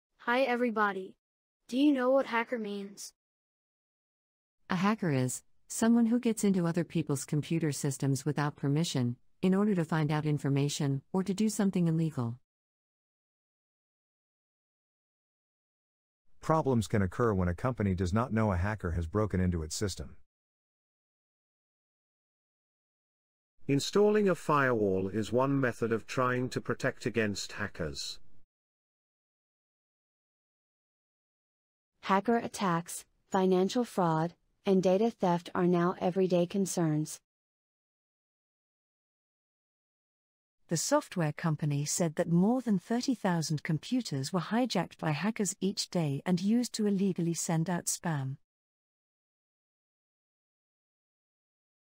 Do you know what HACKER means? --- Learn English with Kevin 😁😉
Do you know the meaning of HACKER? Here are some examples for you.